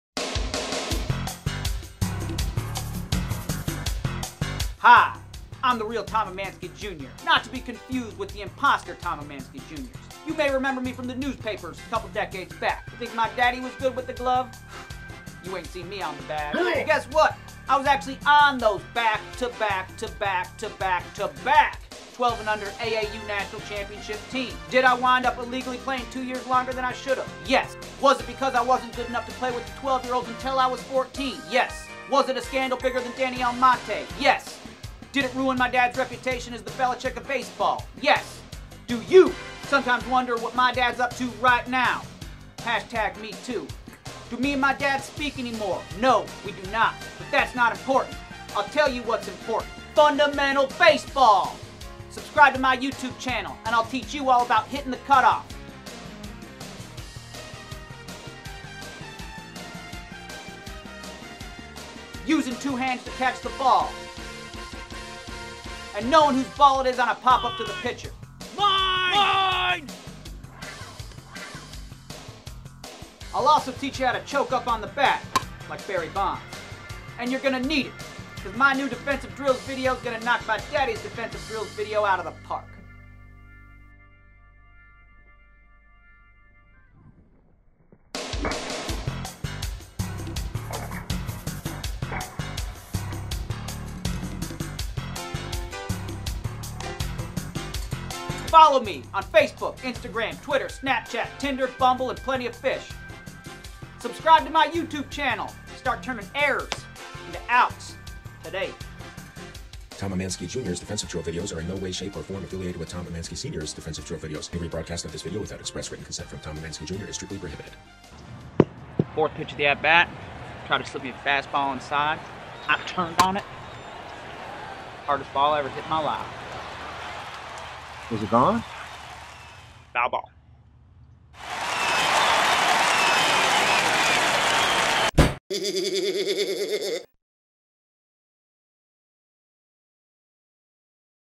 Tom Emanski Jr. INFOMERCIAL - BASEBALL GALAXY
In honor of MLB opening day (2019) we are world premiering BASEBALL GALAXY with TOM EMANSKI JR.

A never before seen inside look at a child phenom as a grown man, teaching fundamentals just like his old man.